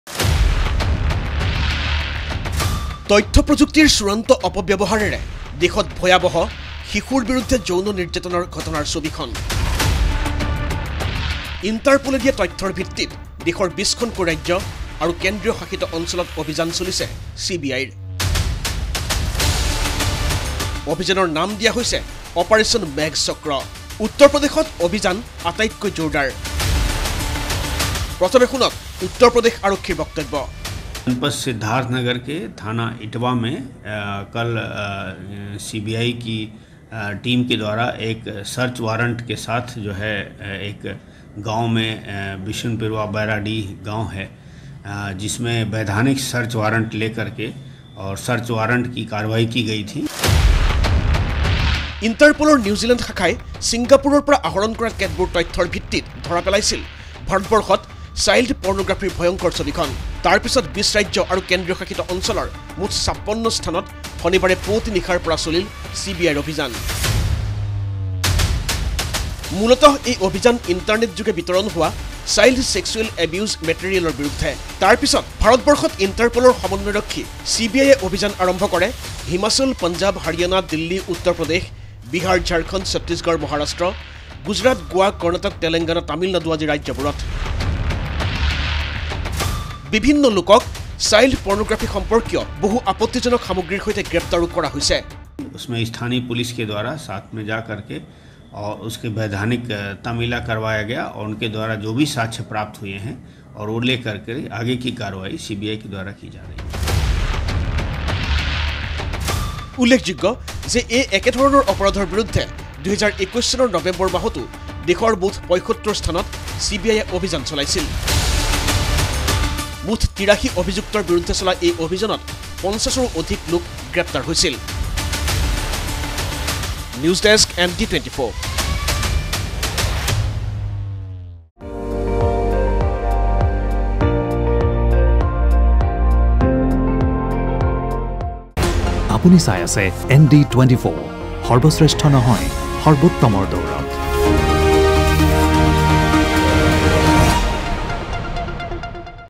এইবাৰ CBI কঠোৰ চাইল্ড পৰ্ণগ্ৰাফীৰ বিৰুদ্ধে । ইণ্টাৰপলৰ তথ্যৰ ভিত্তিত এই অভিযান
এইবাৰ CBI-এ জাল মেলিছে দেশৰ চাইল্ড পৰ্ণগ্ৰাফী প্ৰস্তুত কৰা অসাধু চক্ৰটোৰ বিৰুদ্ধে। স্বয়ং ইণ্টাৰপলে যোগান ধৰা চাঞ্চল্যকৰ তথ্য। এই তথ্যৰ ভিত্তিত ২০ ৰাজ্যৰ ৫৬ স্থানত শনিবাৰে পুৱতি নিশাৰে পৰা চলিছে CBIৰ অভিযান। গ্ৰেপ্তাৰ হৈছে বহুলোক।